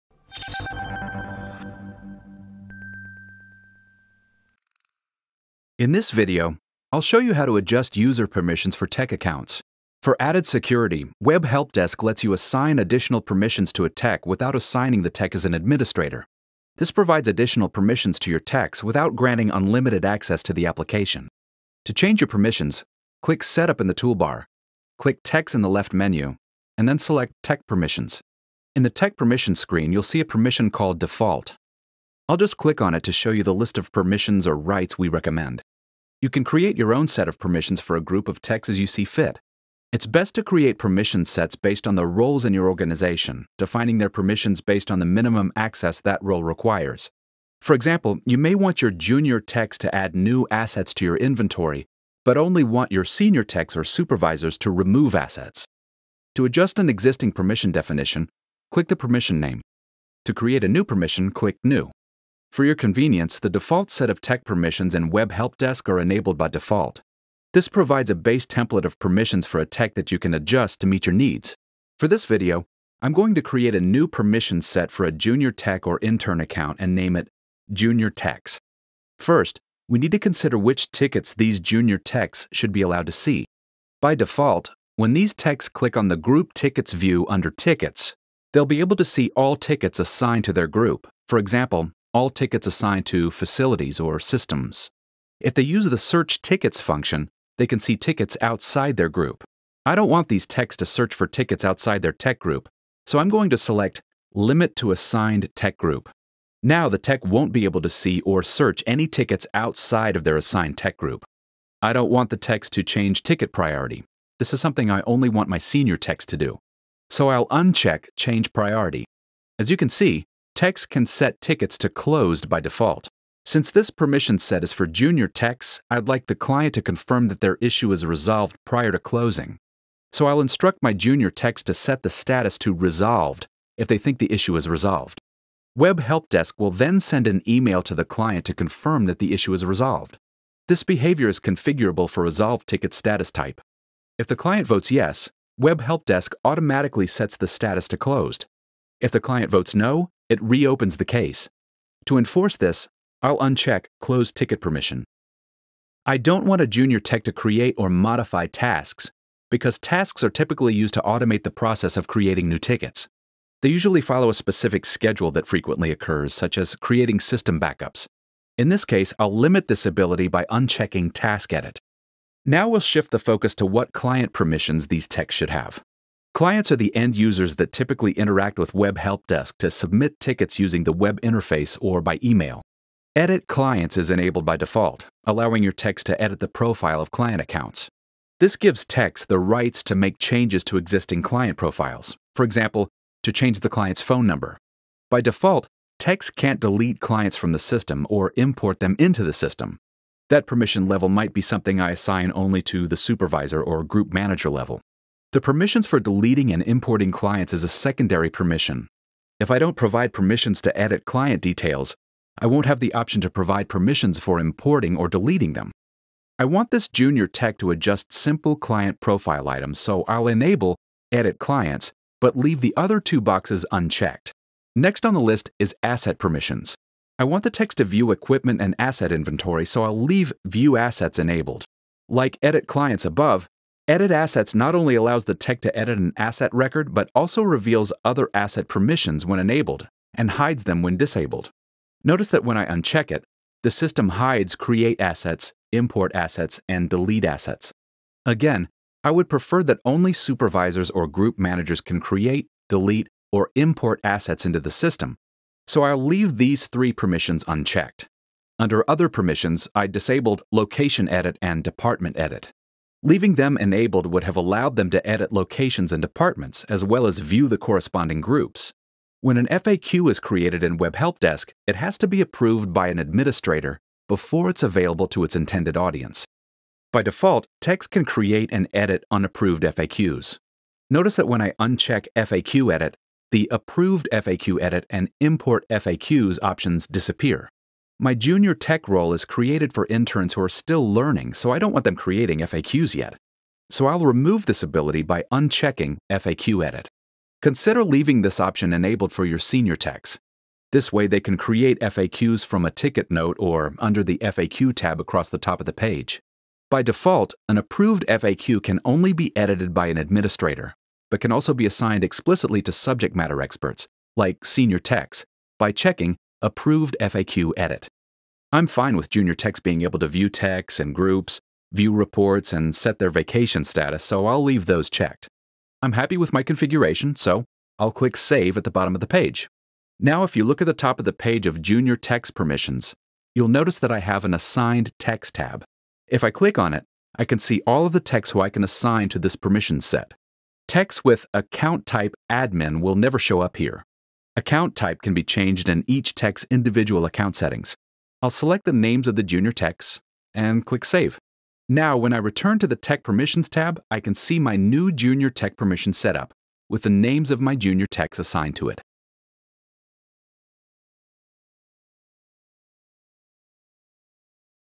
Web Help Desk Training: Setting up Permissions
This video demonstrates how to adjust user permissions for tech accounts in the Web Help Desk software.